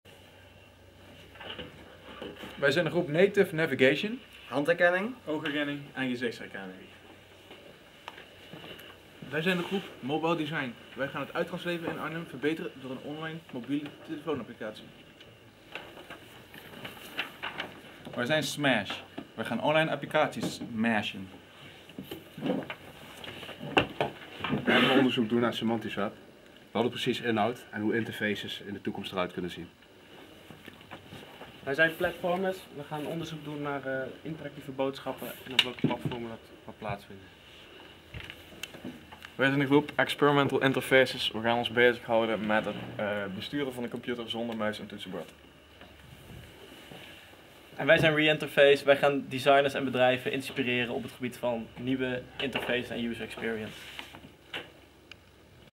Minor DMP
Overzicht van alle groepen en wat ze gaan onderzoeken tijdens deze minor.